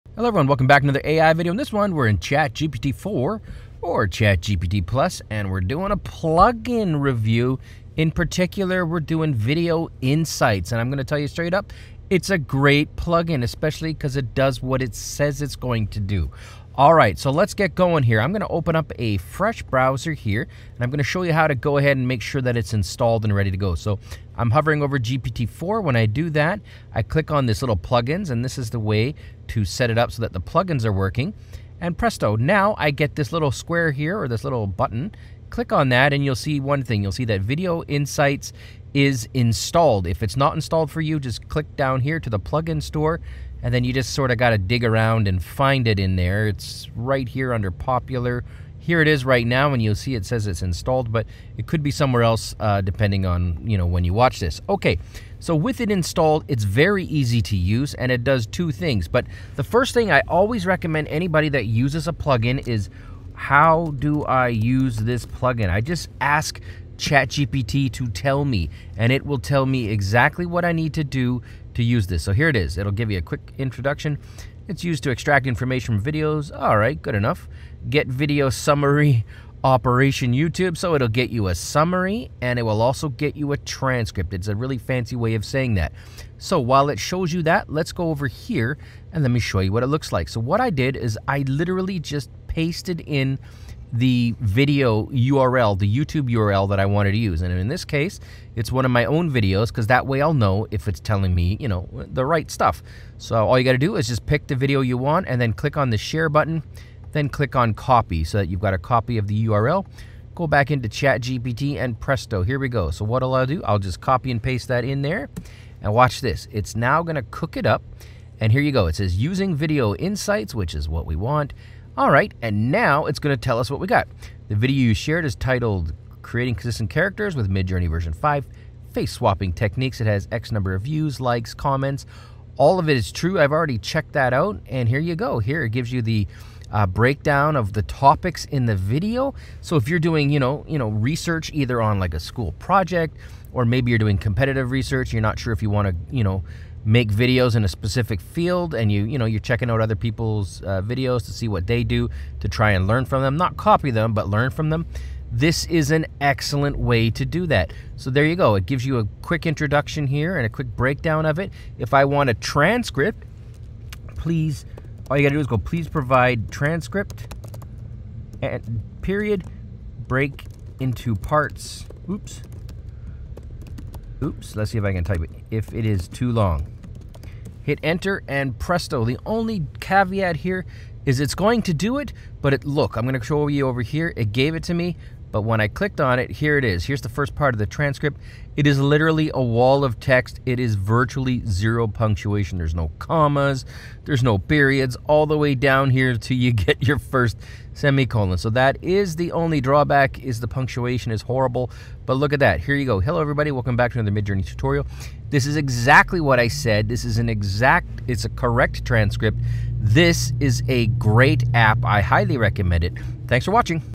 ChatGPT Video Insights Plugin Review - Get Detailed Summaries, Transcripts, and Competitive Insight!
In today's video, we're diving deep into the ChatGPT plugin Video Insights, specifically exploring its powerful features for video insights.🔔

We kick off at 00:23 with a brief overview of the plugin, and I show you how to install and set it up on your browser. Stick around to understand the easy-to-navigate interface that helps you extract maximum value from your videos.

Next, at 00:46, I guide you through the process of locating the Video Insights plugin in the store. It's often listed under the "popular" section, but depending on when you're watching this, it could be located elsewhere.

At 01:05, I emphasize the importance of understanding how to use any plugin. With ChatGPT, it's as simple as asking! Get quick introductions, video summaries, and transcripts - all at your fingertips.

At 01:26, I demonstrate what the plugin looks like in action. By simply pasting a YouTube URL into the plugin, it works its magic. As a real-world test, I use one of my own videos.

By 01:58, I walk you through the step-by-step process of copying a video URL from YouTube and pasting it into the plugin. What follows is an impressive breakdown of insights, including video title, number of views, likes, and comments.

At 02:14, I highlight one of the most powerful features of the ChatGPT plugin - its ability to provide a comprehensive breakdown of video topics. This feature is invaluable for research purposes, whether it's for academic projects or competitive analysis.

At 02:35, I show you how to request a transcript from the plugin. However, it's important to note that the transcript provided is a continuous text block with minimal punctuation, which I illustrate at 03:18. But rest assured, the transcript is accurate and a valuable resource.

Stick around till the end for my final thoughts on the ChatGPT Plugin. It's a game-changer, and I highly recommend it to anyone looking to extract actionable insights from their videos. 👍